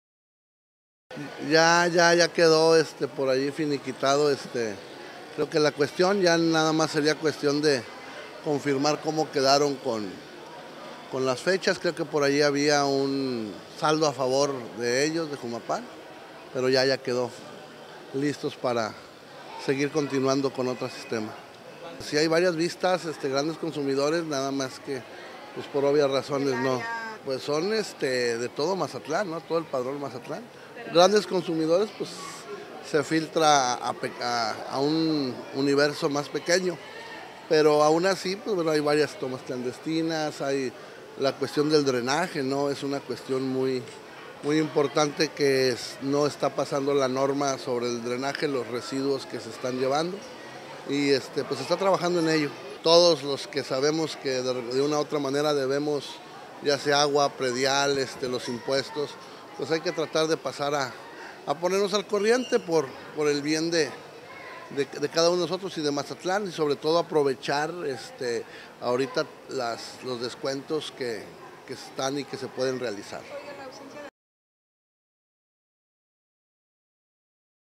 Totalmente regularizada empresa Espectáculos Costa del Pacífico
El Secretario del Ayuntamiento, José de Jesús Flores Segura, dio a conocer que la empresa Espectáculos “Costa del Pacífico” ya realizó el pago completo de la desudas pendientes que tenía en Oficialía Mayor